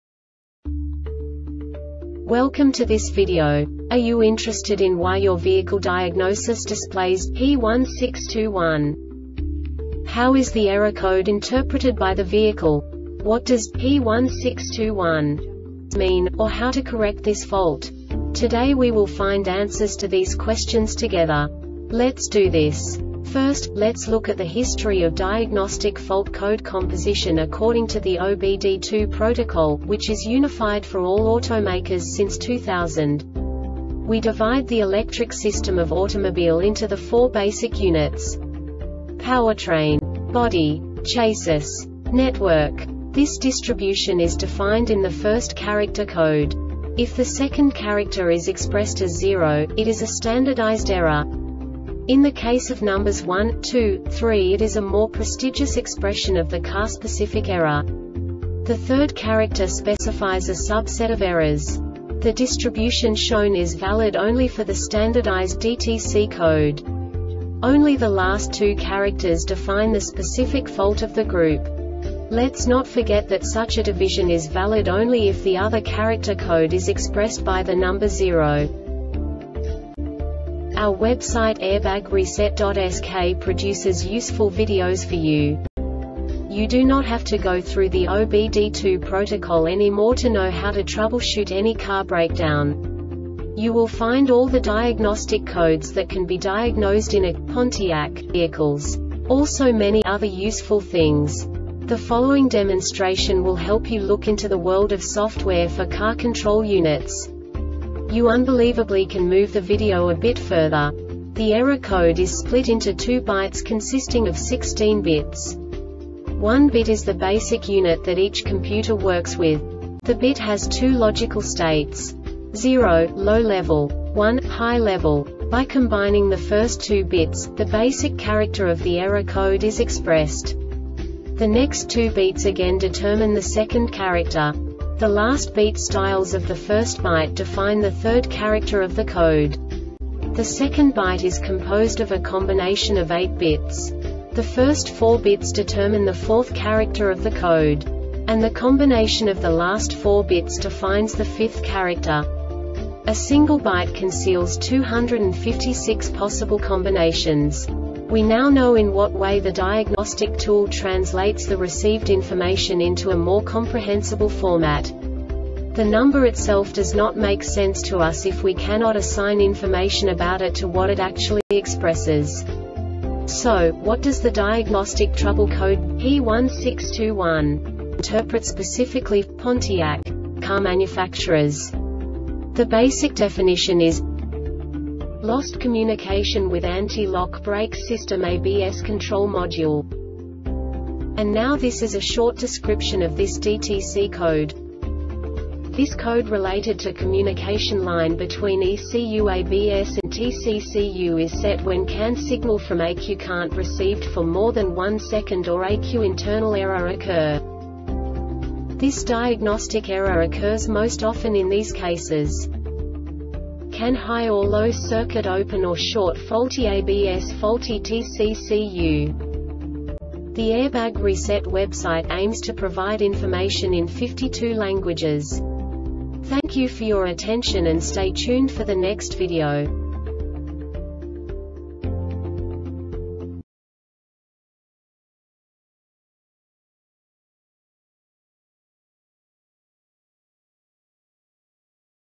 DTC Pontiac P1621 Short Explanation
Contents:
0:21 Basic DTC analysis according to OBD2 protocol standard.
1:48 Insight into programming
2:58 A diagnostically understandable description of the Diagnostic Trouble Code
3:05 Basic error definition

Make:
Pontiac
Code:
P1621
Definition:
Description:
This code related to communication line between ECU(ABS) and TCCU is set when CAN signal from ECU can’t received for more than 1sec or ECU internal error occur.
Cause:
 CAN HIGH or LOW circuit open or short. Faulty ABS. Faulty TCCU .